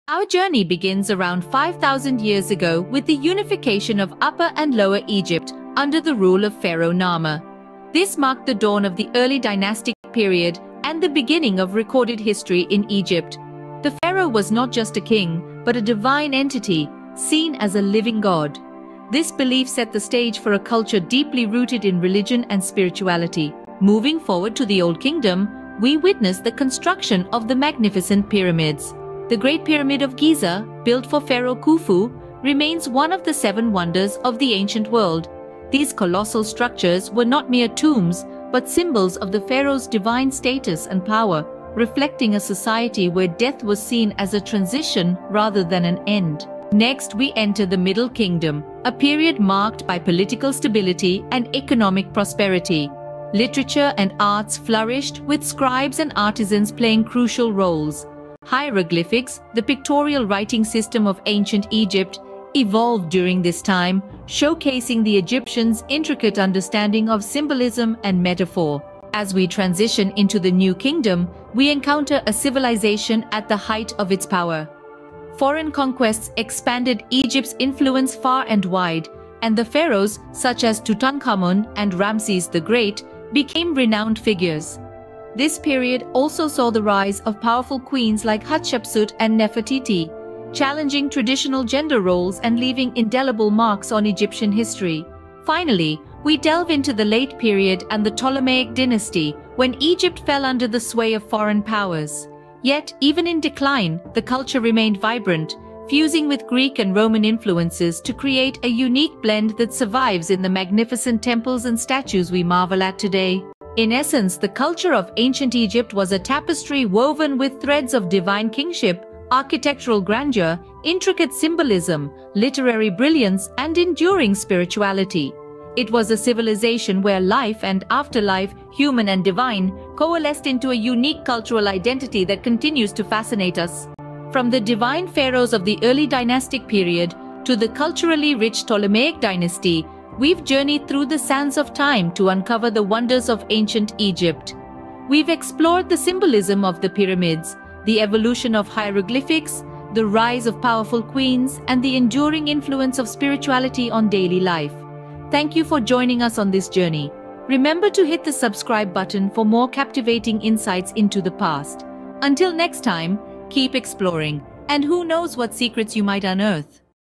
Unraveling the Wonders of Ancient Egypt: From Divine Pharaohs to Powerful Queens
Join us on an awe-inspiring journey through the sands of time as we explore the rich history and cultural marvels of ancient Egypt. From the magnificent pyramids to the rise of powerful queens, uncover the secrets of this intriguing civilization.